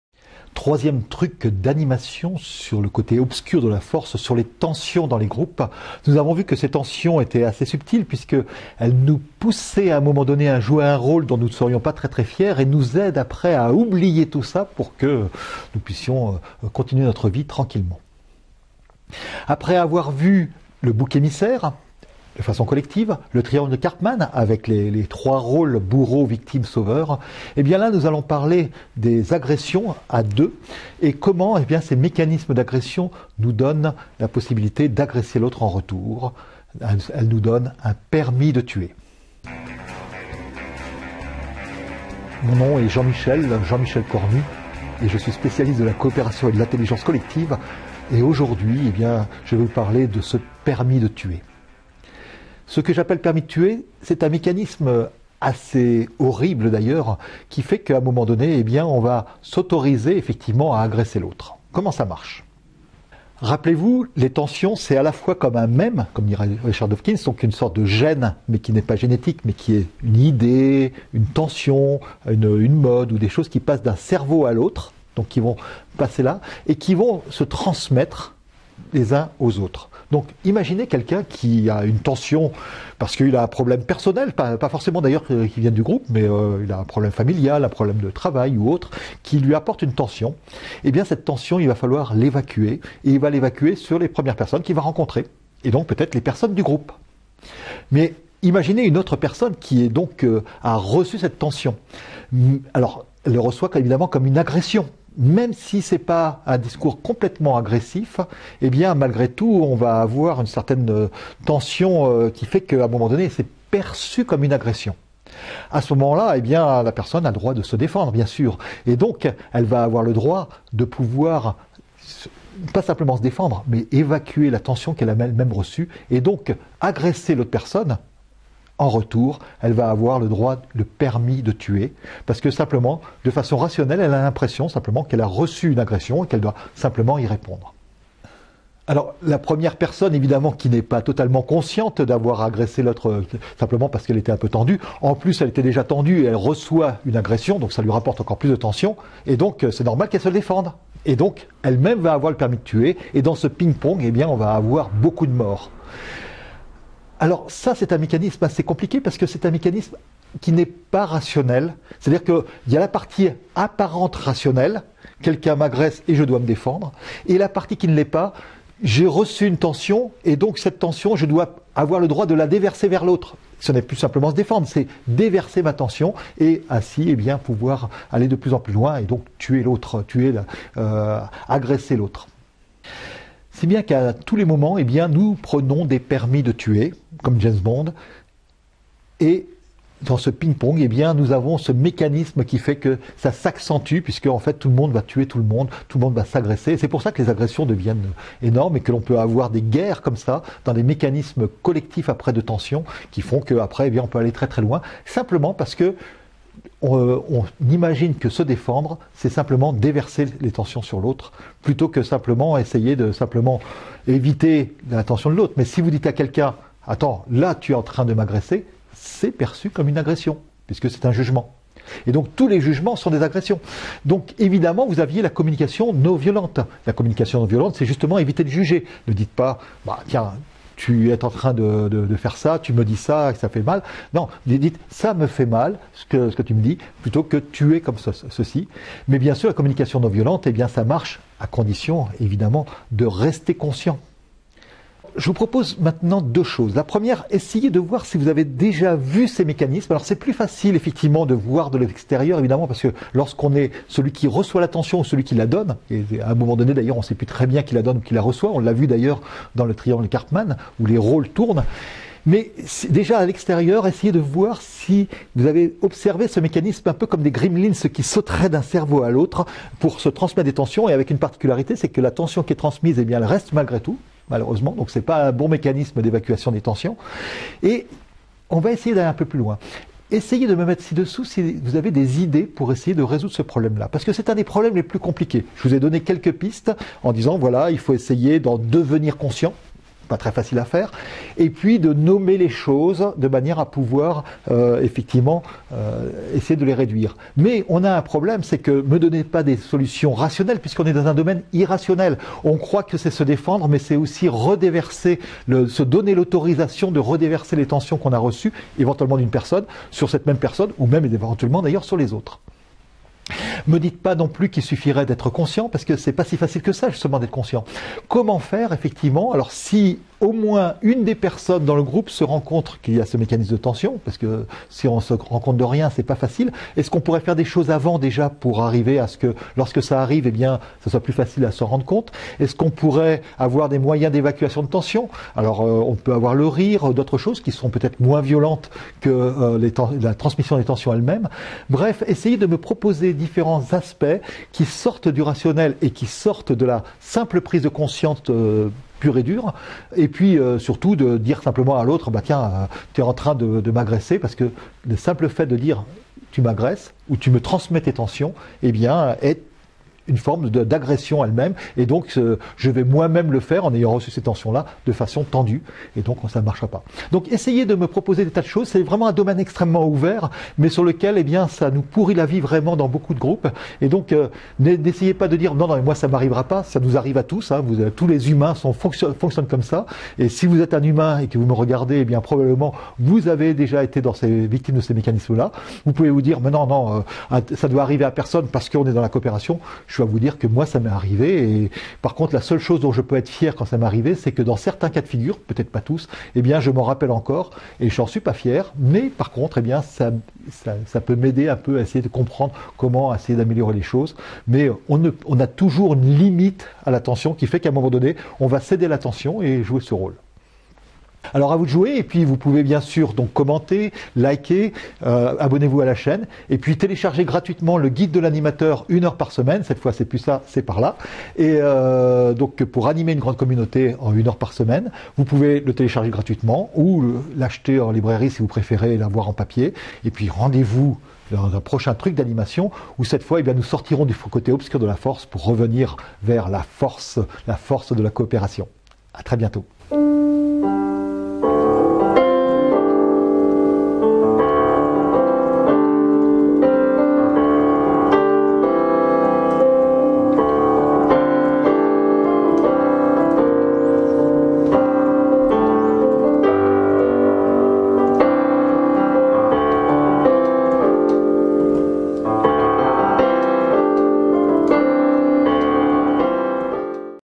Le permis de tuer
Troisième volet de la série sur les tensions dans les groupes avec cette fois un mécanisme où les tensions non seulement se propagent mais s’amplifie. Sous un couvert très rationnel “j’ai le droit de répondre quand je me sens agressé” se déroule un mécanisme inconscient “et je vais en profiter du coup pour évacuer toutes mes tensions…”